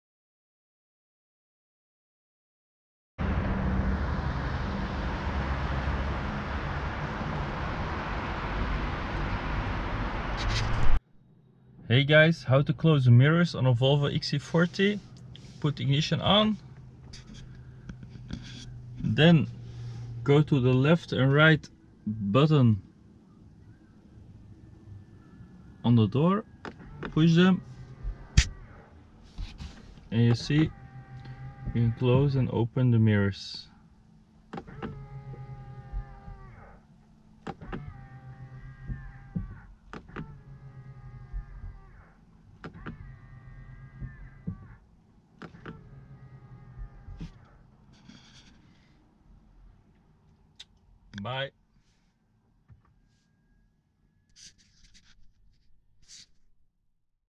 Volvo XC40 close mirrors when ignition is on ( hidden function) ⚙
Volvo xc40 close mirrors with hidden trick

please 👍 or discribe  for more Volvo XC 40 tips and tricks